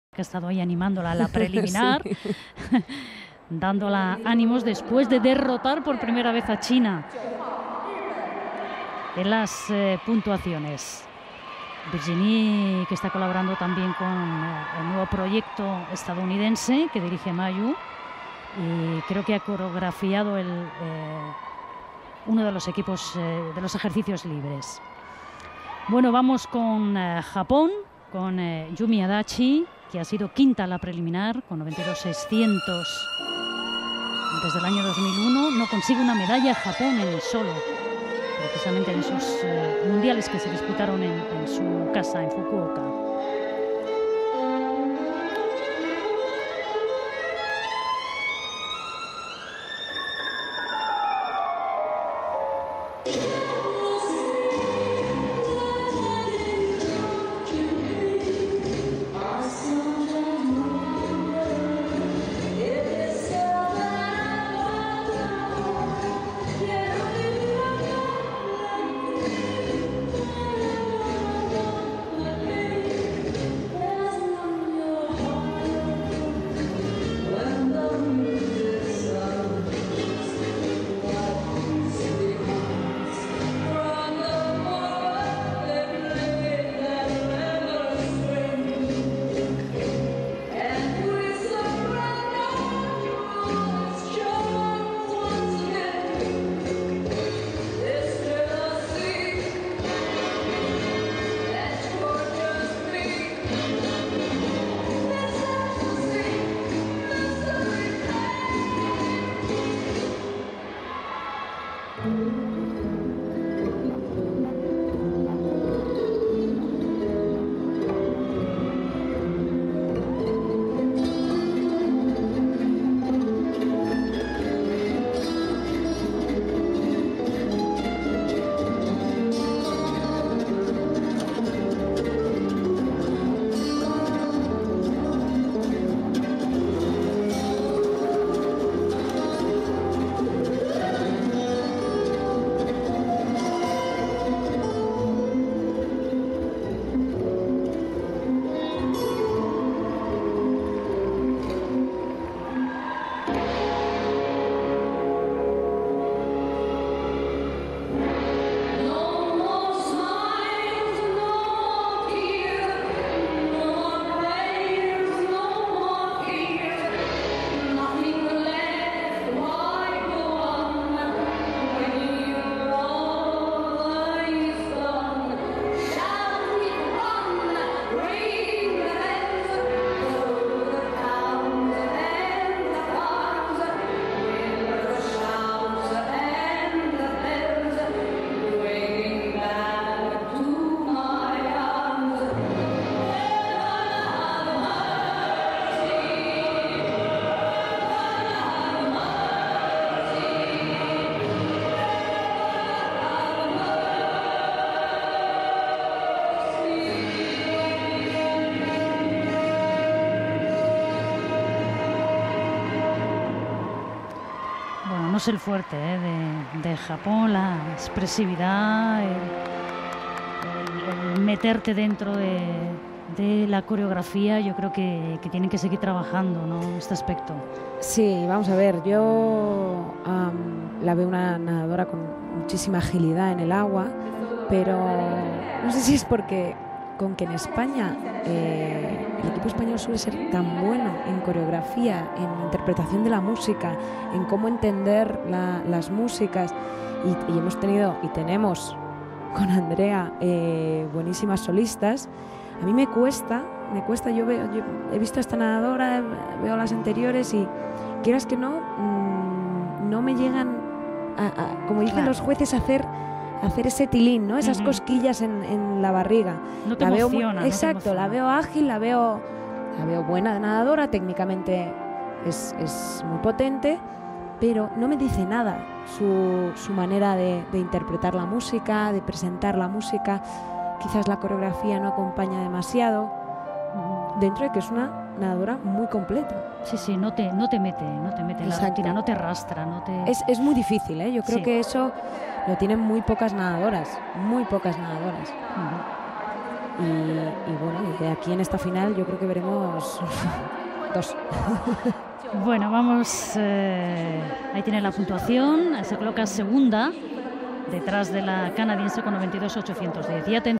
Mundial Natación 2011 Final Solo Libre JAPÓN Yumi Adachi
Rutina de solo libre de Yumi Adachi, representando a Japón, en los Mundiales de natación 2011, disputados en Shanghai.